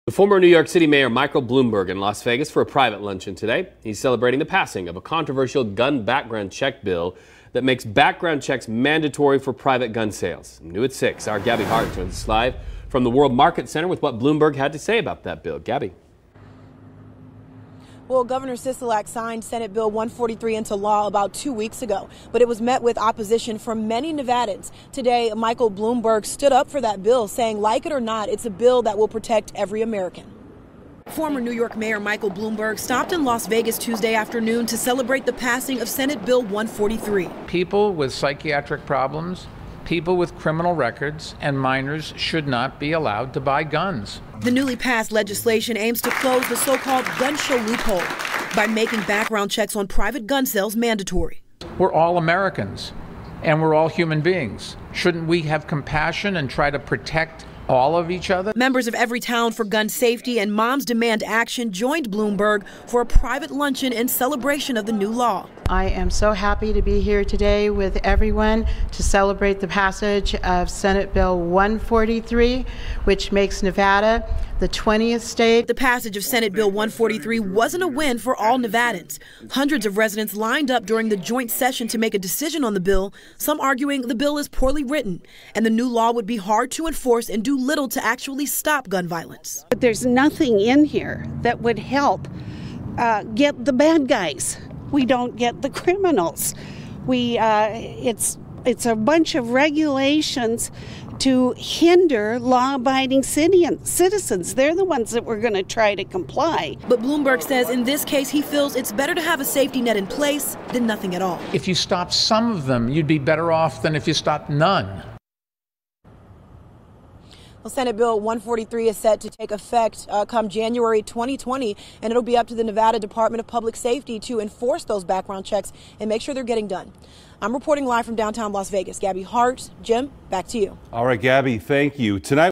Michael Bloomberg stops in Las Vegas to celebrate passing of gun background check bill
Former New York Mayor Michael Bloomberg stopped in Las Vegas, Tuesday afternoon, to celebrate the passing of Senate Bill 143.
"People with psychiatric problems, people with criminal records, and minors should not be allowed to buy guns," said Bloomberg.
The newly passed legislation aims to close the so-called "gun show loophole" which makes background checks on private gun sales mandatory.